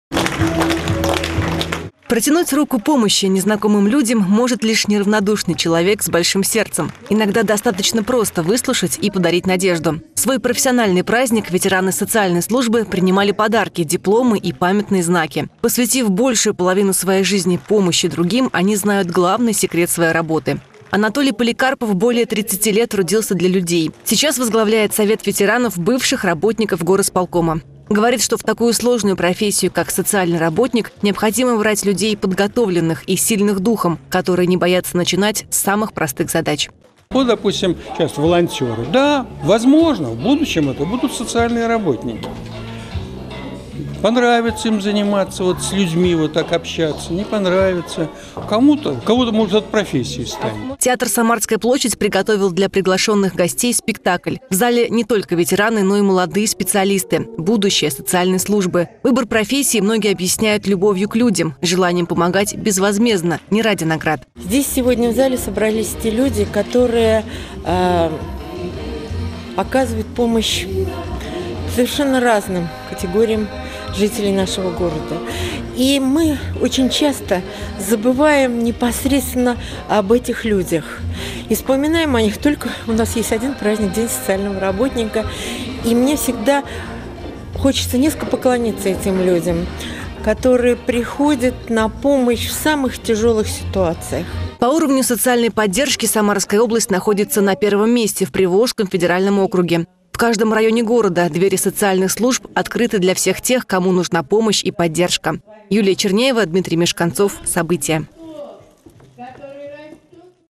В Самаре наградили ветеранов социальной службы
Верны своему призванию. В Самаре продолжаются праздничные мероприятия в честь Дня социального работника. Ветеранам социальной службы сегодня вручили грамоты, памятные знаки и подарки. Многие из них посвятили профессии всю жизнь